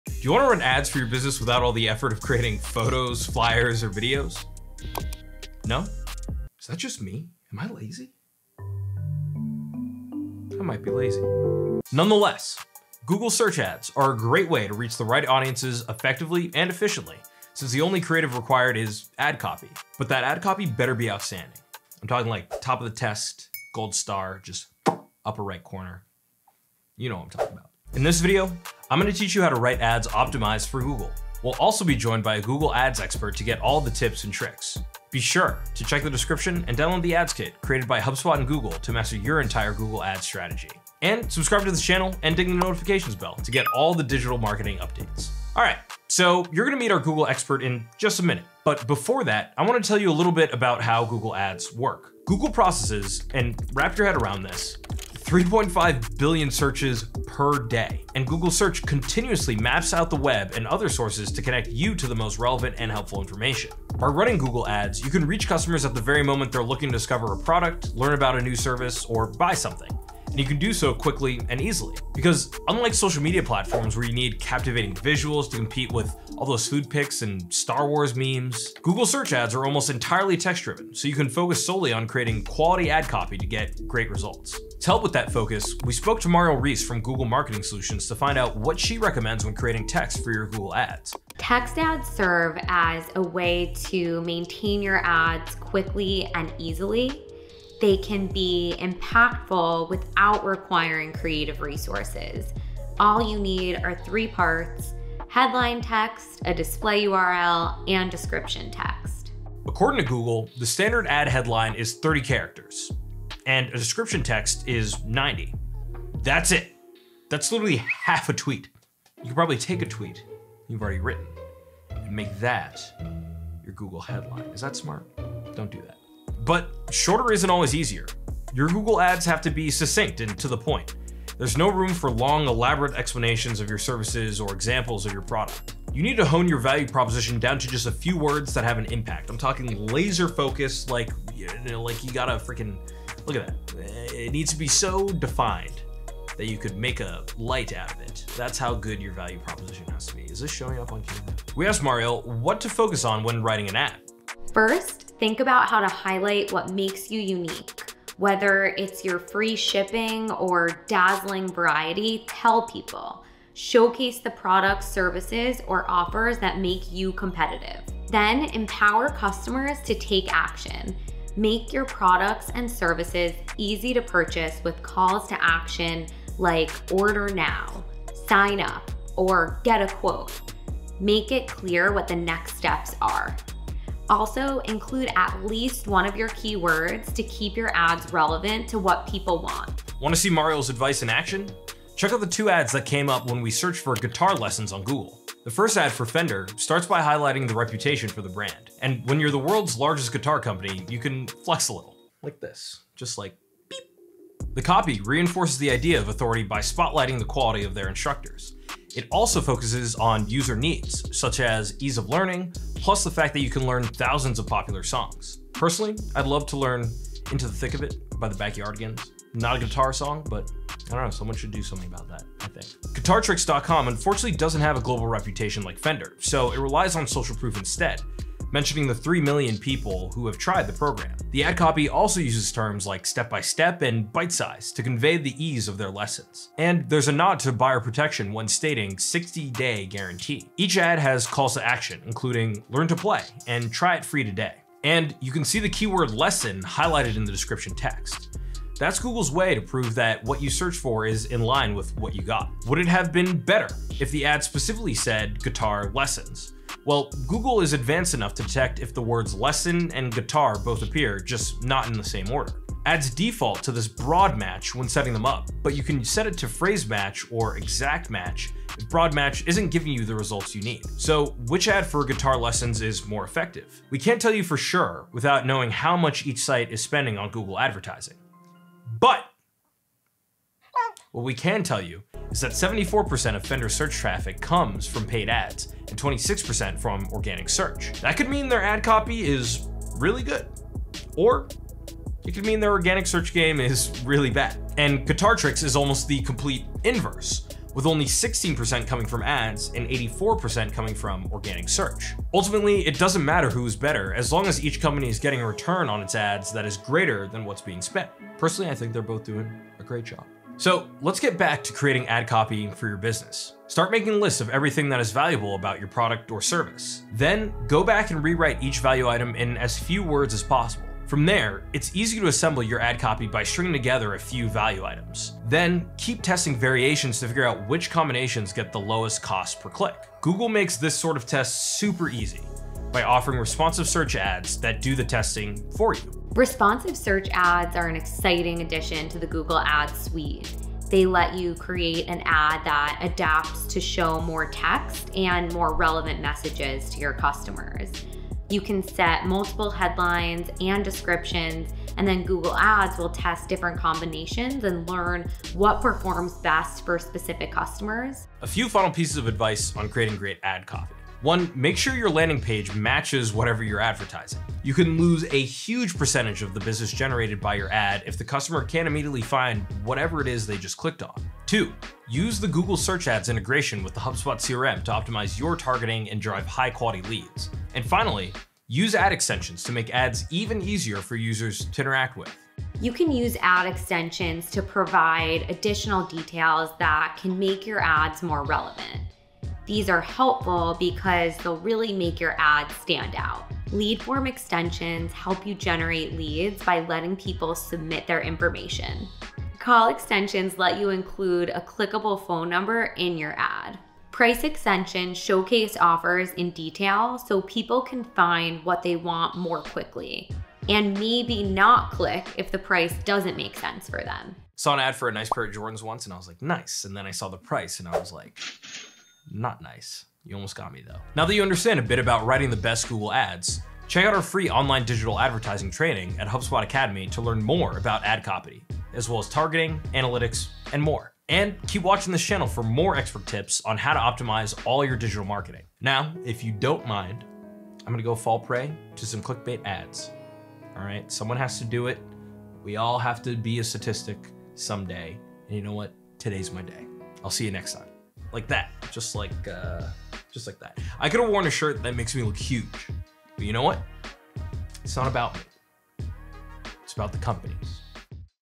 How To Write Amazing Ad Copy | Google Expert Reveals Top Tips (2022)
Learn how to write the best ad copy and how To Use Google Ads with this kit

The secret to getting the clicks is in the ad copy. But it's not just about writing good content, you need to create ad copy that captures your audience and makes them want to click on your ads. In this video, we’re going to teach you how to write ad copy optimized for Google and we even spoke to an expert at Google to get all the tips and tricks.

📔 Favorite Free Certification Courses:
• Digital Marketing Certification:

Video chapters:
0:00 intro
0:45 How Google ads work
2:30 Ad copy Step 1- Highlight what makes you unique
2:48 Ad copy Step 2 - Easy to purchase
3:02 Ad copy Step 3 - Keywords
4:15 Keyword match
5:09 How to create ad copy